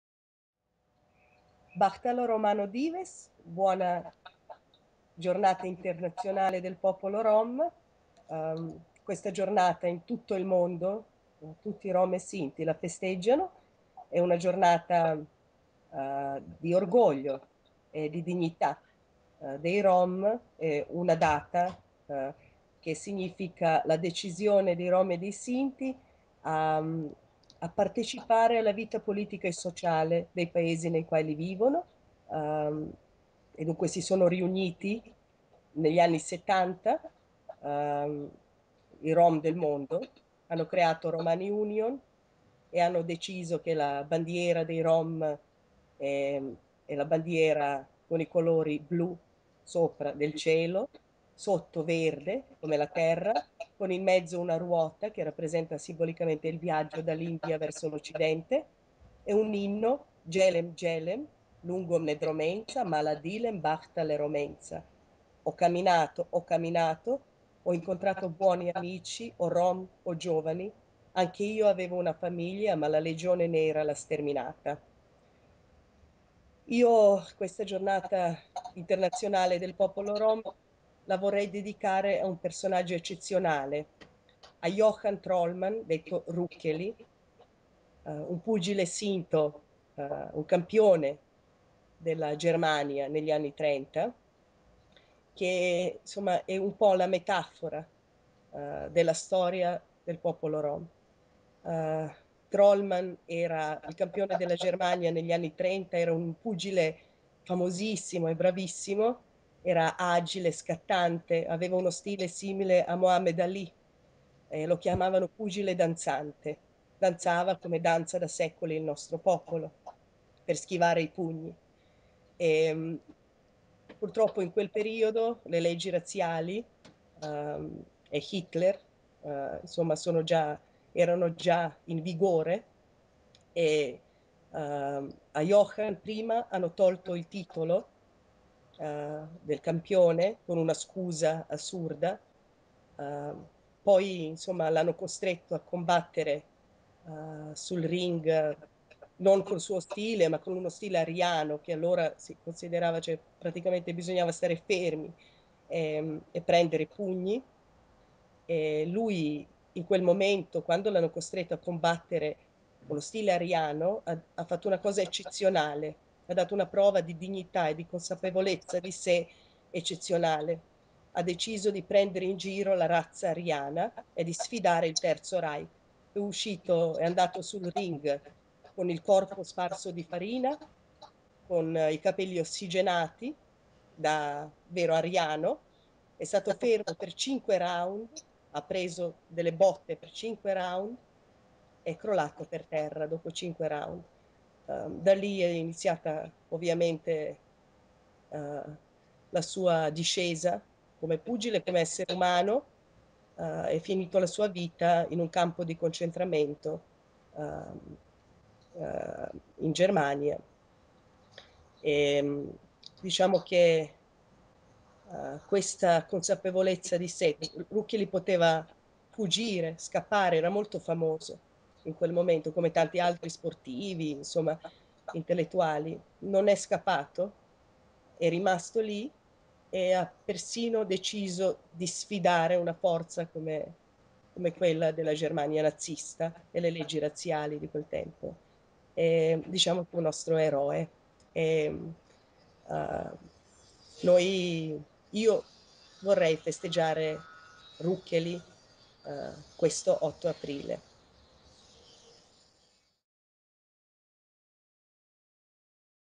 Dijana Pavlovic per la Giornata Internazionale della minoranza Rom
L'8 aprile si celebra la giornata internazionale della minoranza Rom e con l'occasione l'Ambasciata americana vuole ricordare la comunità Rom che in tutto il mondo vive ai margini della società e affronta quotidianamente la discriminazione e il razzismo.  Per sostenere il rispetto dei loro diritti abbiamo voluto dare voce a Dijana Pavlovic, una giovane Rom che nel 2011 ha partecipato ad un programma di scambio culturale del Governo americano.